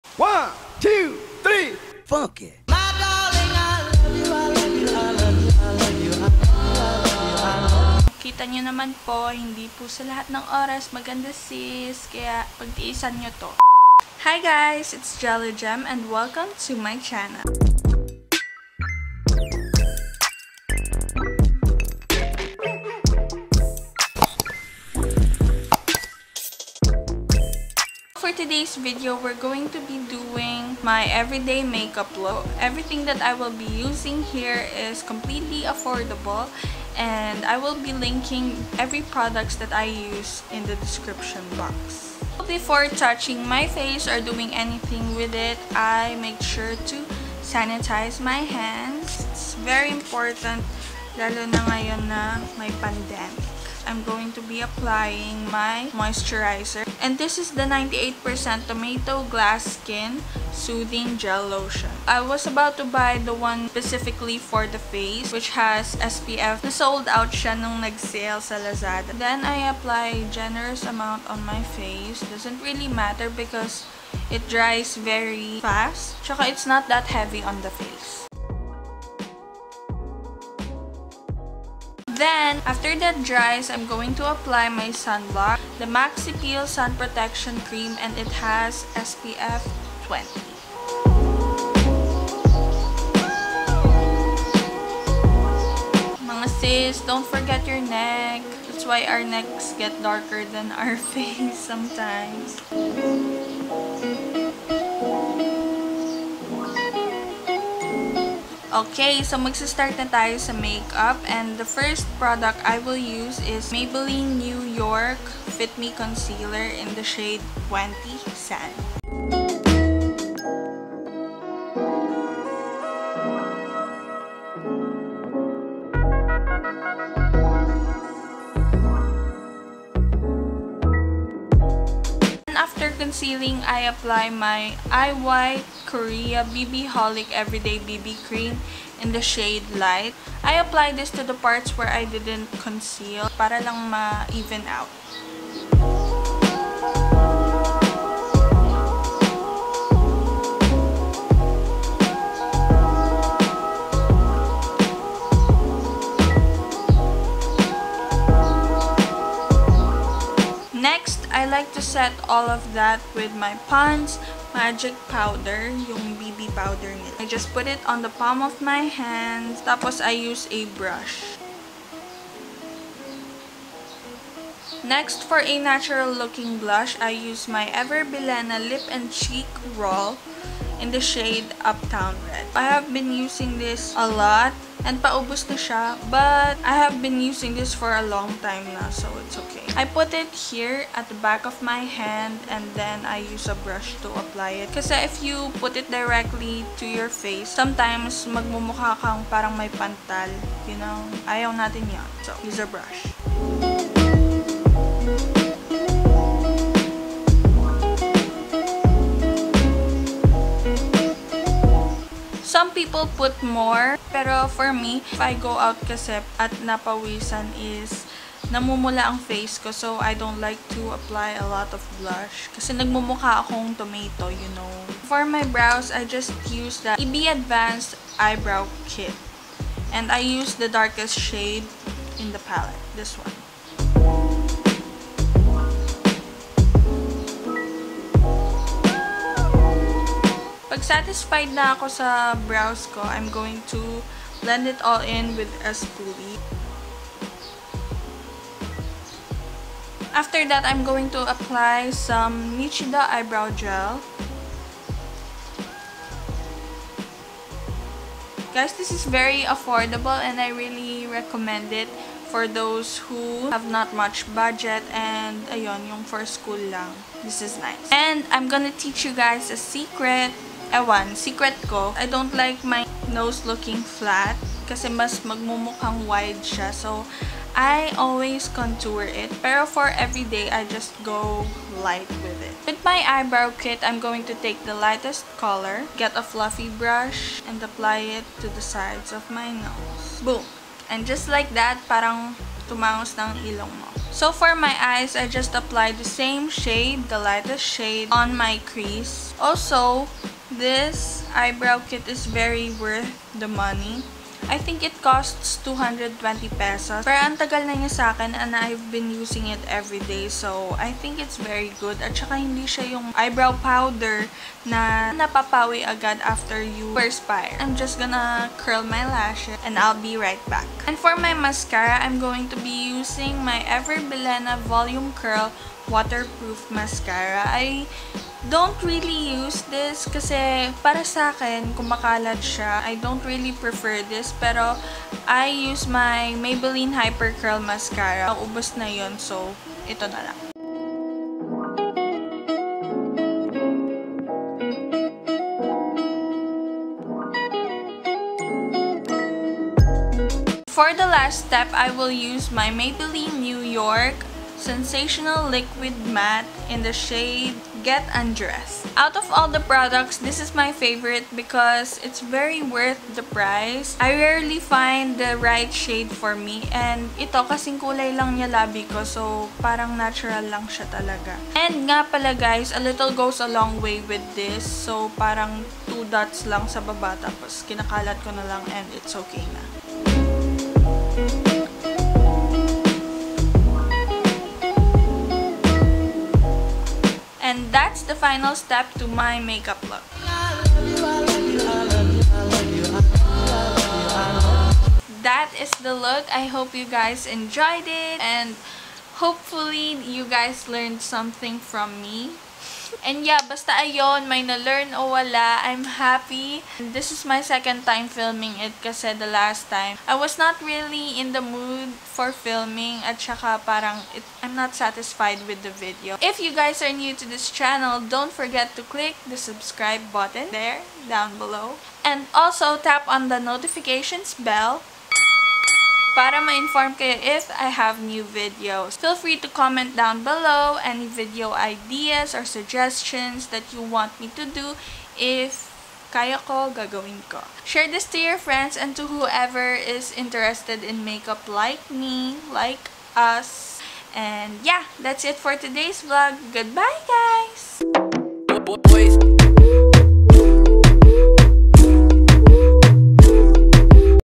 Affordable Everyday Makeup Look 2020 (all under Php 250)
✨Products Used✨

Eb Insta curl mascara (sorry guys couldnt find the one I actually used in the vid)